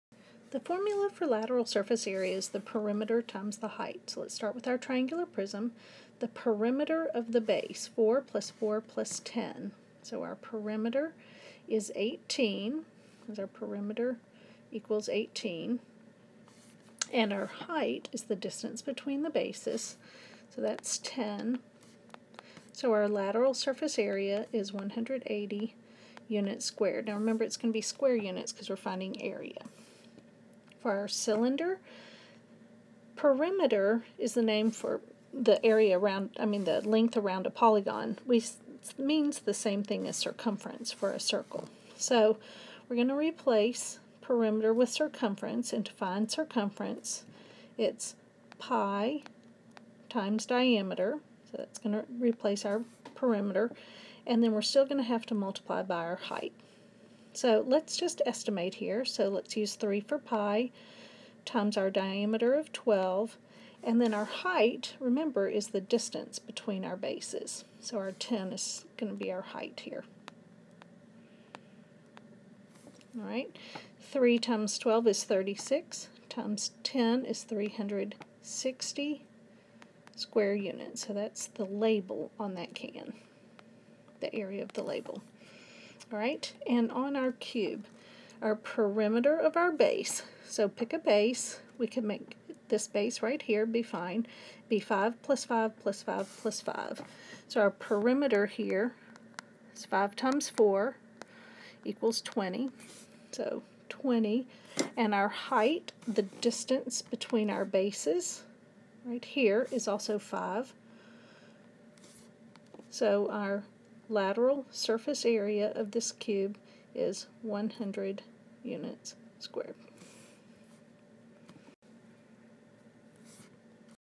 Lateral Surface Area of Prisms and Cylinders
Calculate Lateral Surface Area using LSA=Ph, including Triangular Prisms and Cylinders.